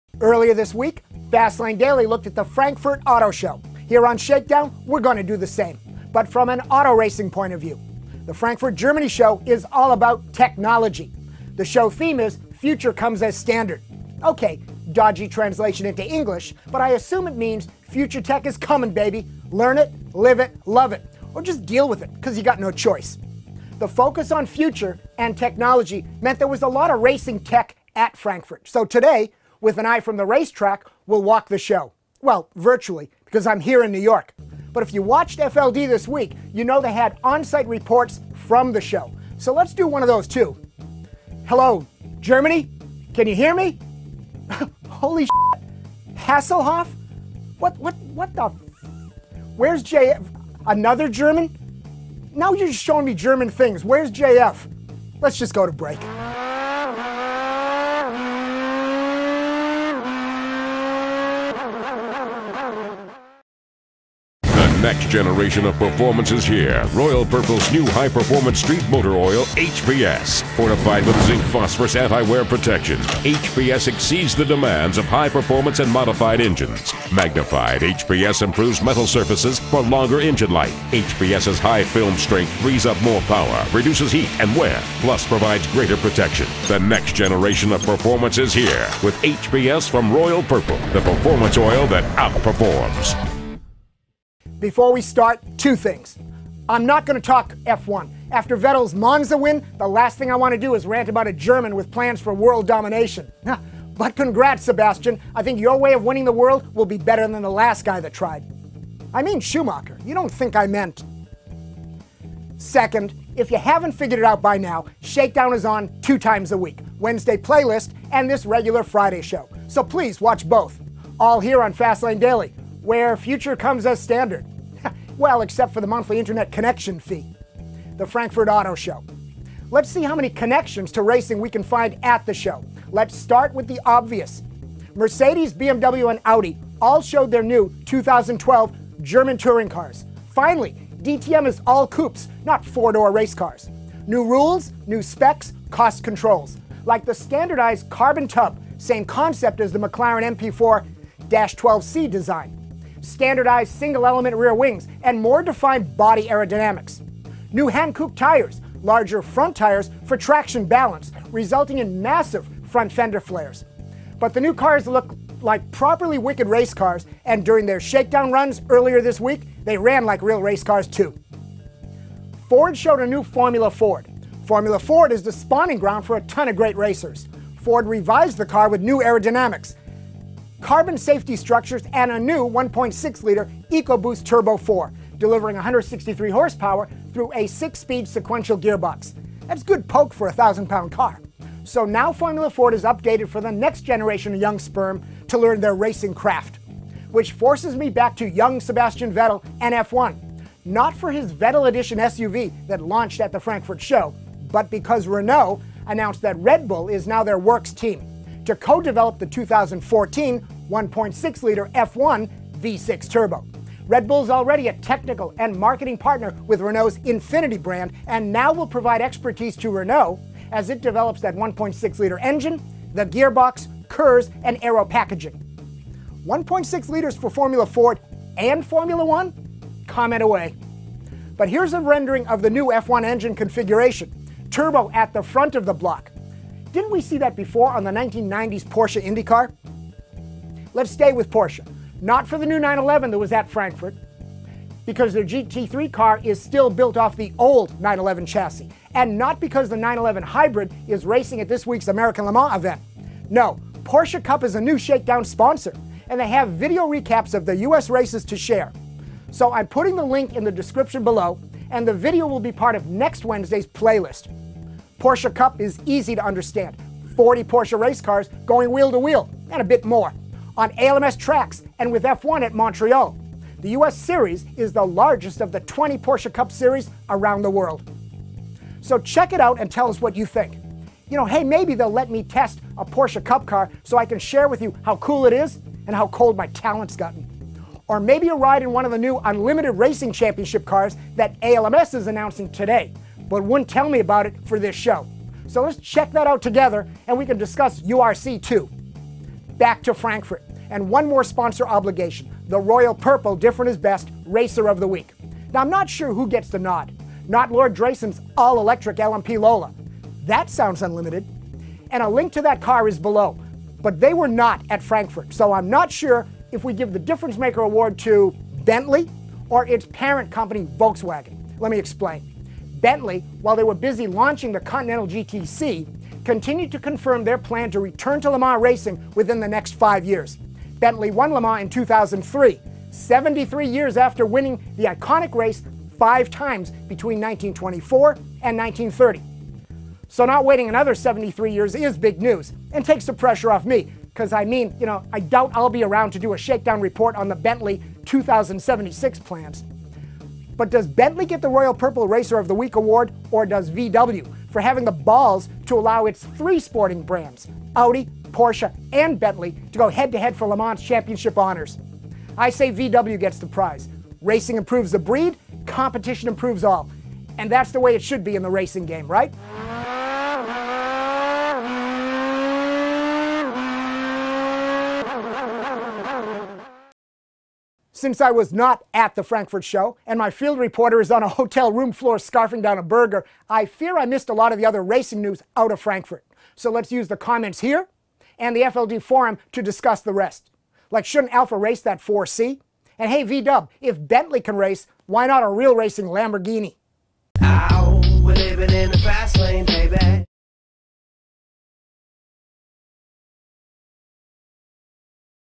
Auto Racing at the Frankfurt Show - ShakeDown
Leo Parente looks at the race cars at the Frankfurt Auto Show. Mercedes, Audi, BMW launch their DTM cars. Ford freshens Formula Ford. Renault and Red Bull partner up in F1. ShakeDown introduces it's new sponsor - IMSA Porsche GT3 Cup Challenge, by Yokohama. Plus, we call Germany looking for JF Musial.